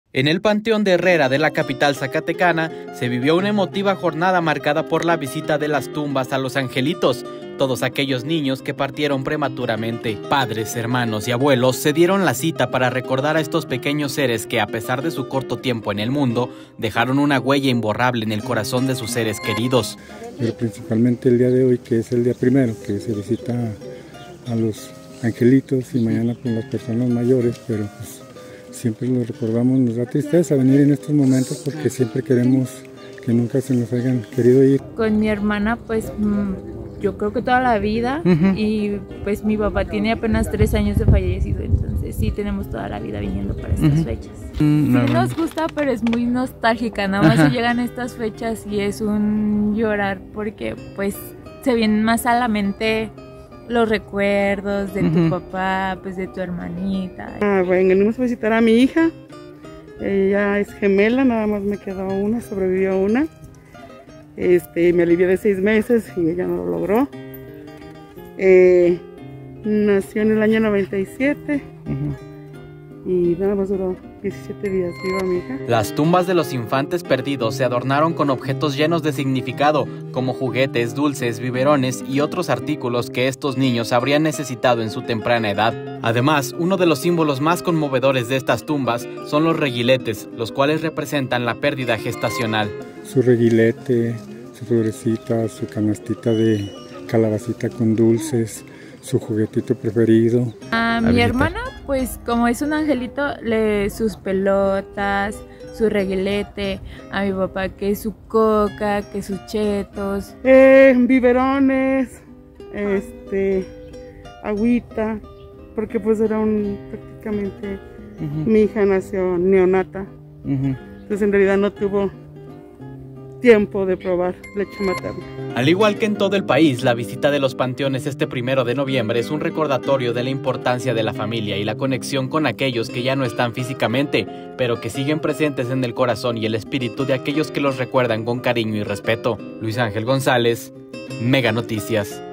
FAMILIARES EN ZACATECAS VISITAN A SUS “ANGELITOS”
En el Panteón de Herrera de la capital zacatecana, se vivió una emotiva jornada marcada por la visita a las tumbas de los "angelitos", todos aquellos niños que partieron prematuramente. Padres, hermanos y abuelos se dieron cita para recordar a estos pequeños seres que, a pesar de su corto tiempo en este mundo, dejaron una huella imborrable en el corazón de sus seres queridos.